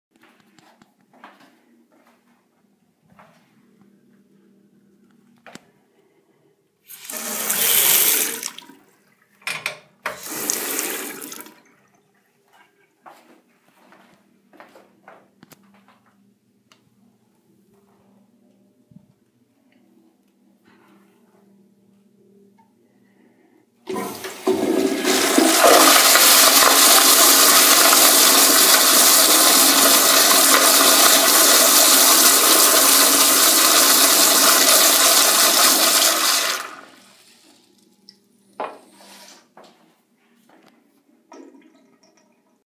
50. Older American Standard Madera toilet #1
On the 1st floor of a courthouse in Lockhart, TX. The building is old, but they sadly remodeled the bathrooms several years ago. Oh well. I really hate these toilets, but this one has a good flush. BTW, each restroom on each floor is right next to the elevator! LOL!!! =D